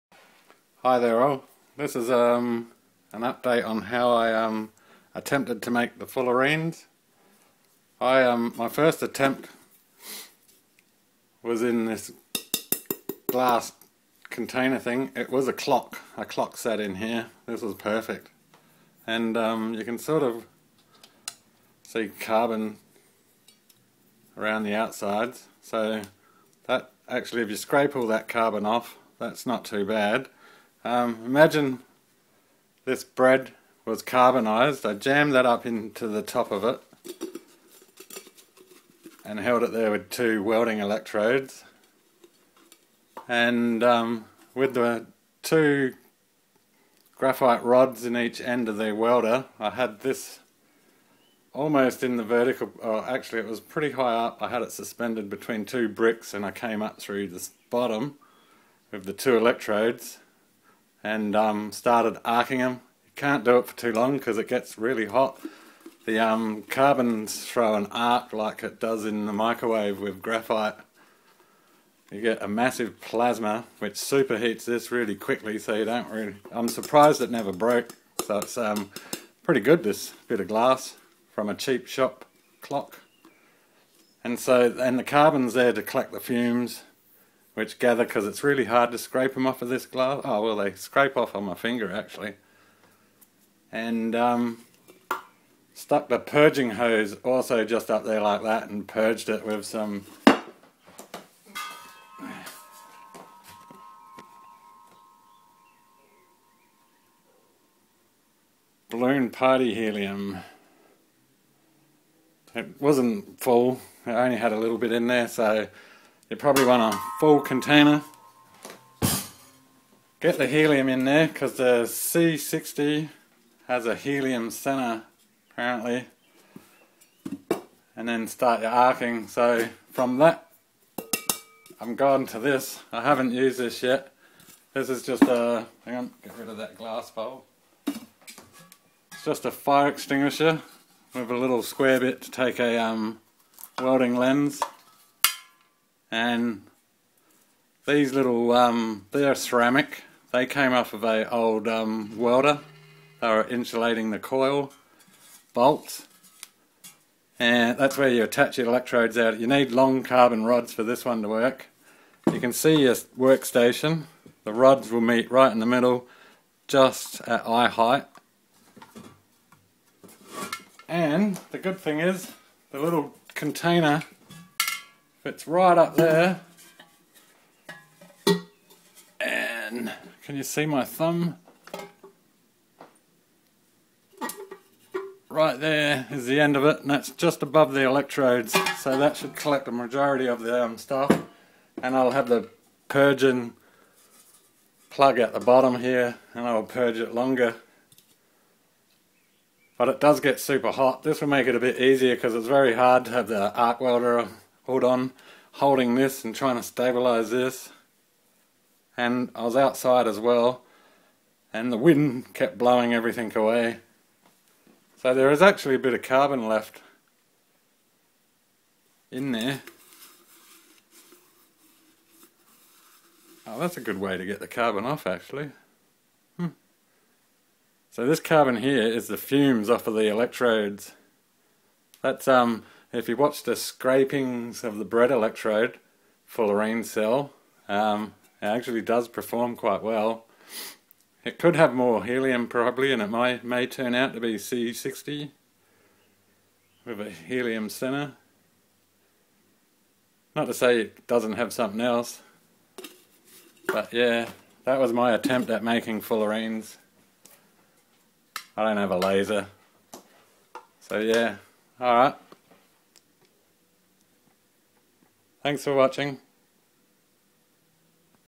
Home Made C60 Carbon
how to maybe make C60 carbon, You may even want to seal the container and pressurise it a bit.